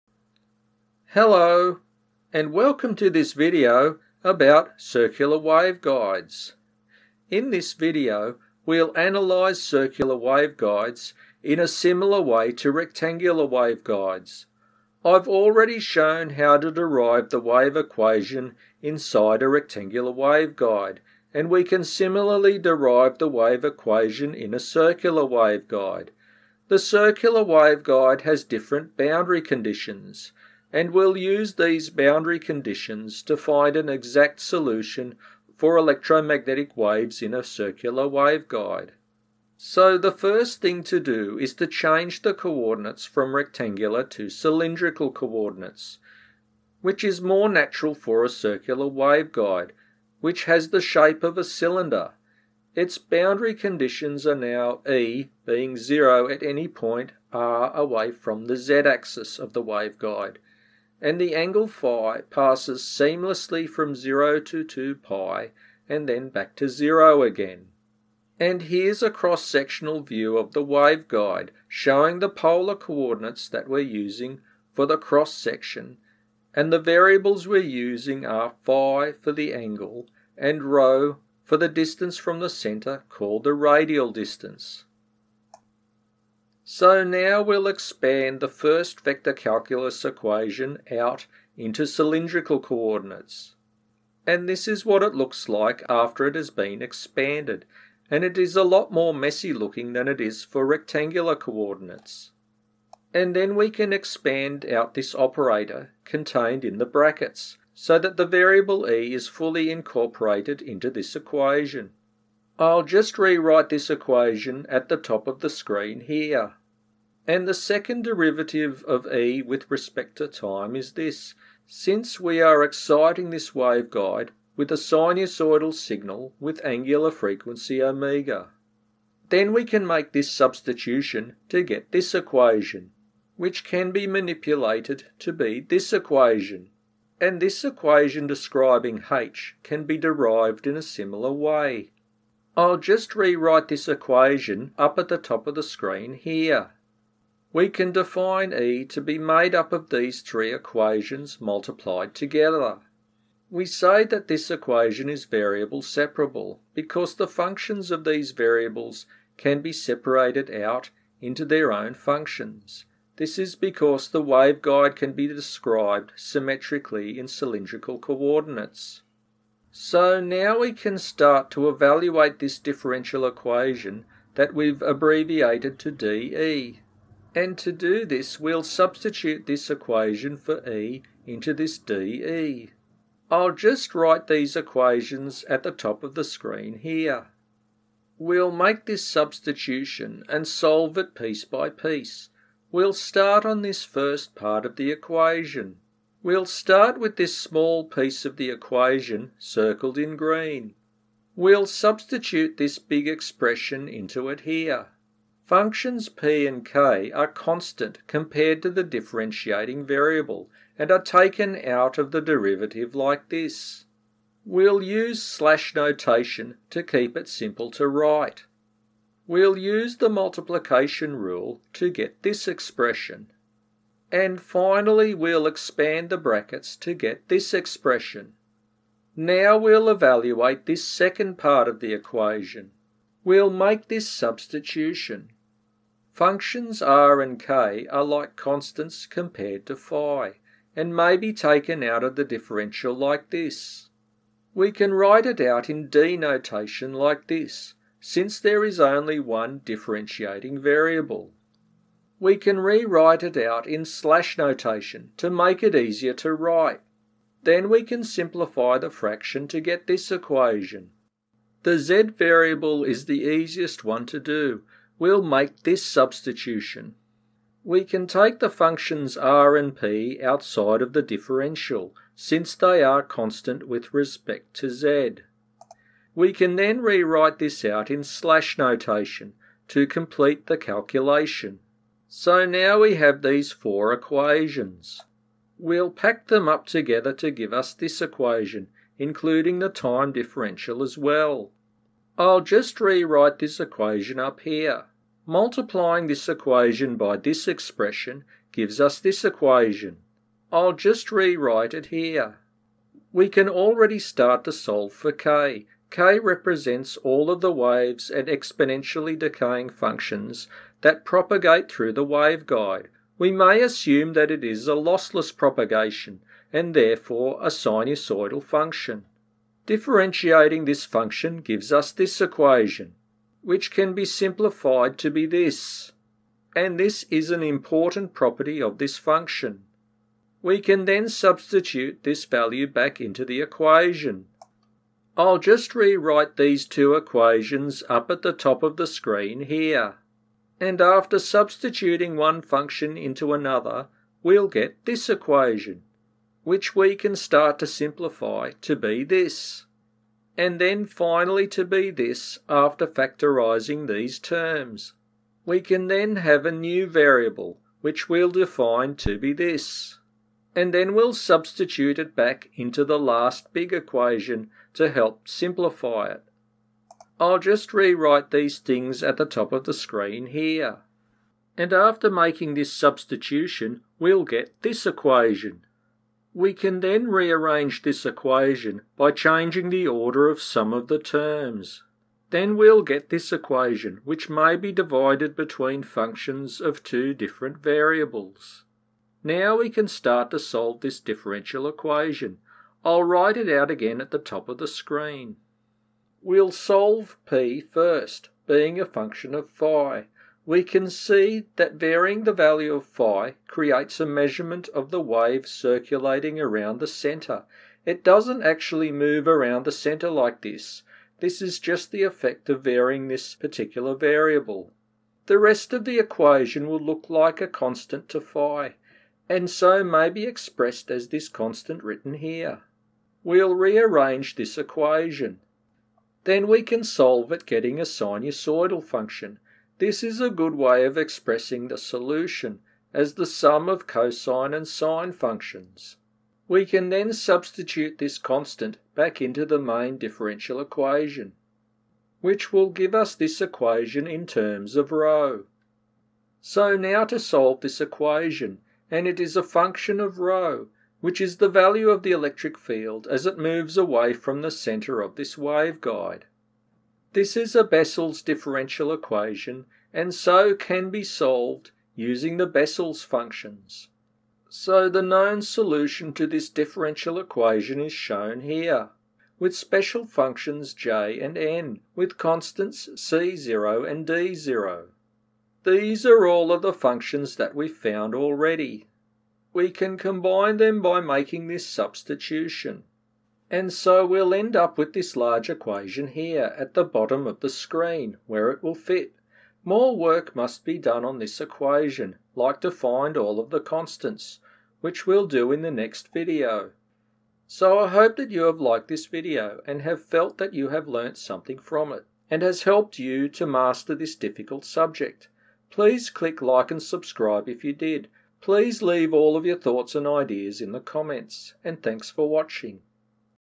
Circular Waveguides p1
A short description of how to derive equations for the circular waveguide. In this first part we'll work with the vector calculus equations in cylindrical coordinates to get some practice with working with them.